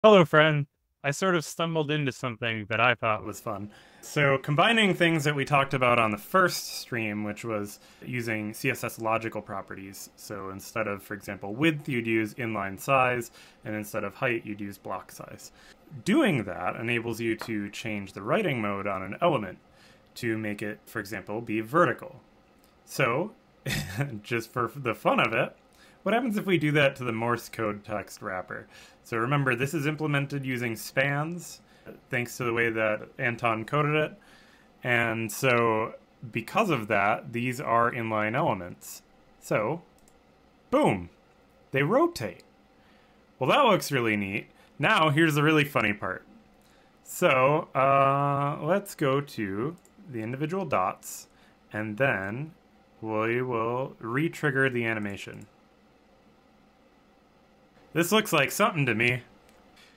Coding with Jon: Experimenting with CSS Logical Properties on Morse Code Text
Catch Jon live 6/21/24 at 12.30 pm EST for another live coding session!

In this fun exploration, we revisit CSS logical properties and demonstrate their effects on a Morse code text wrapper. Watch as inline elements rotate when the writing mode changes and see the fascinating results when individual dots are animated. Join us for this insightful and entertaining experiment!

00:00 Introduction and Fun Discovery
00:03 Exploring CSS Logical Properties
00:25 Applying CSS to Morse Code Text
00:47 Animating the Dots